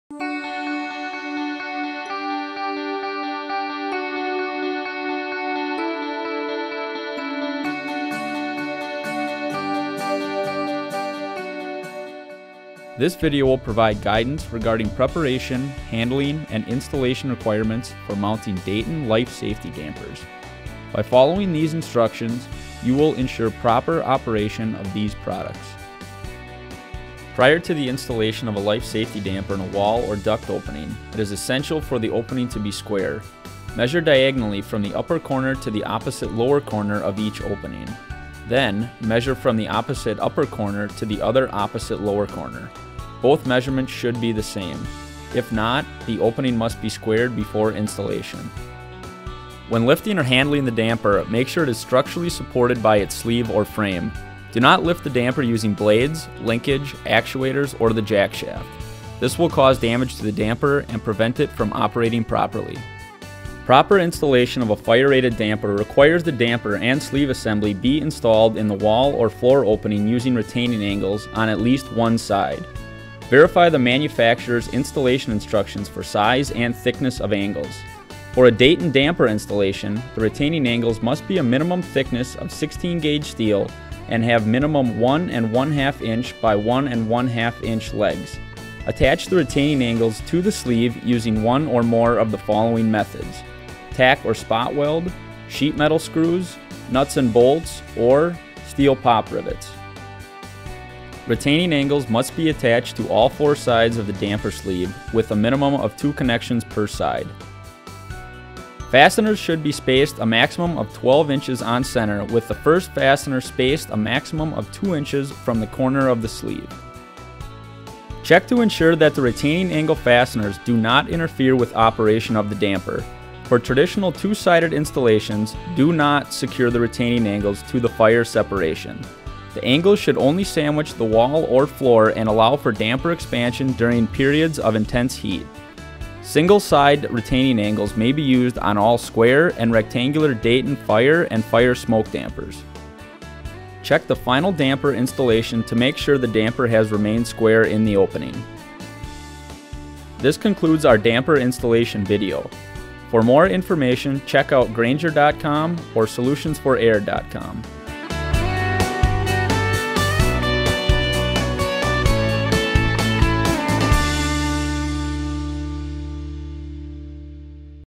Solutions for Air Proper Damper Installation Methods
Solutions for Air Video for Proper Damper Installation

Watch this informative video on proper installation of Dayton fire dampers. Learn tips and tricks to make installation easy and accurate.